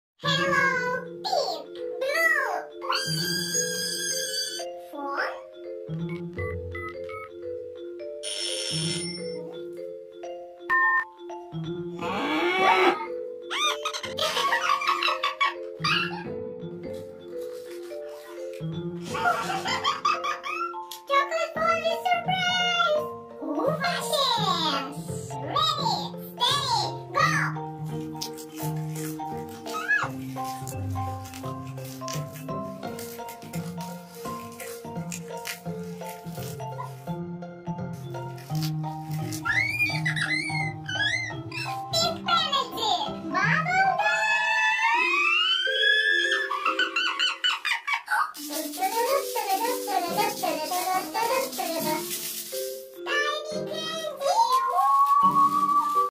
PINK FOOD VS BLUE FOOD
도나와 친구의 빨간색 파란색 음식 챌린지!
 DONA and friend Pink food vs blue food challenge!

재미있고 맛있게 보세요!